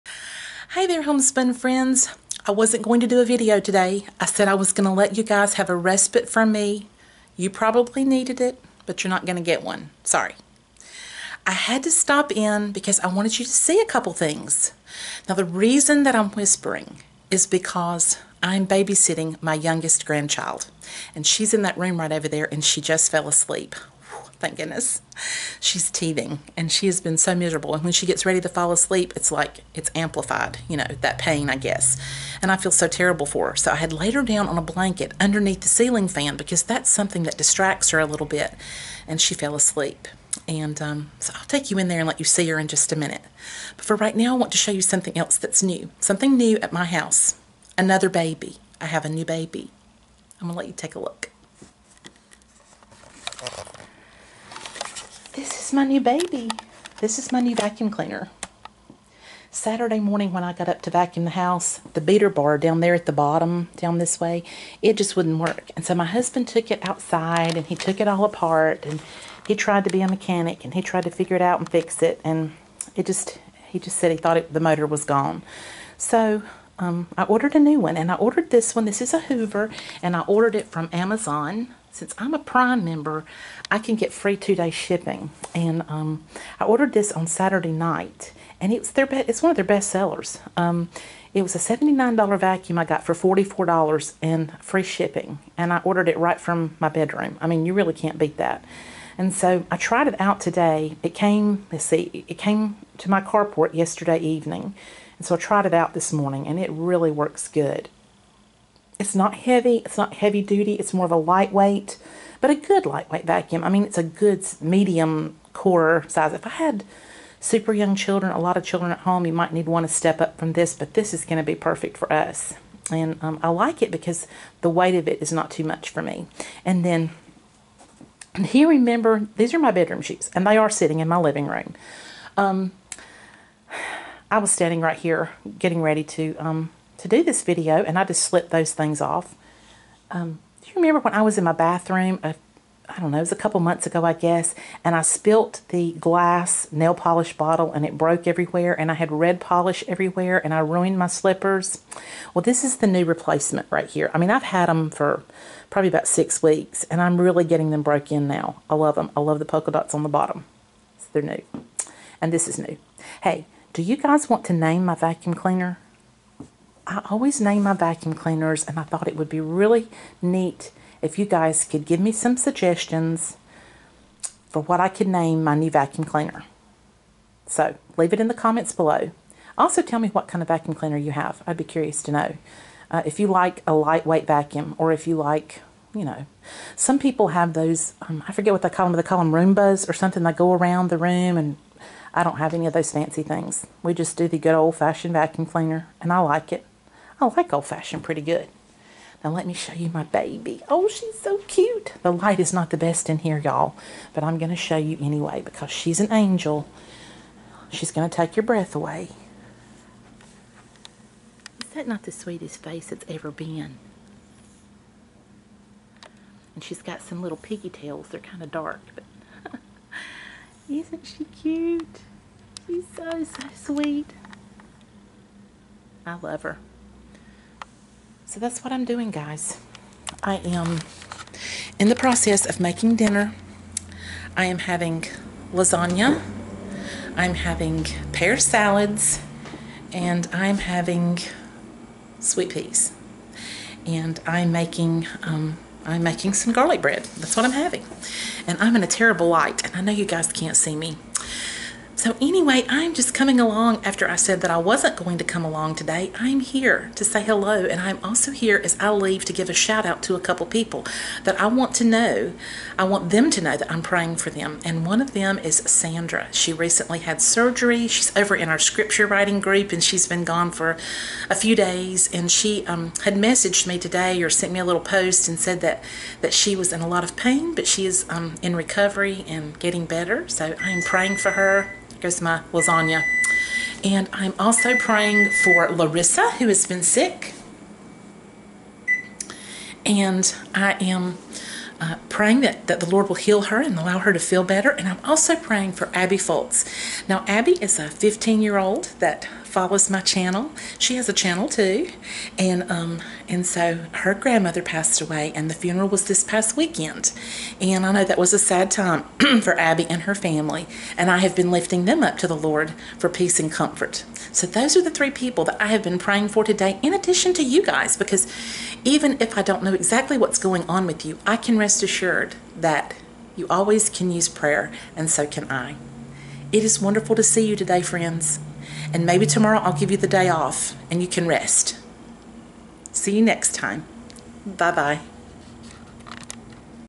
Checking In.....With A Baby!
A busy day ended with me babysitting my youngest grandchild while making dinner...and admiring my latest acquisition.
The thumbnail picture is of little Sunday when she's wide awake..and with her piggy tails let down. LOL.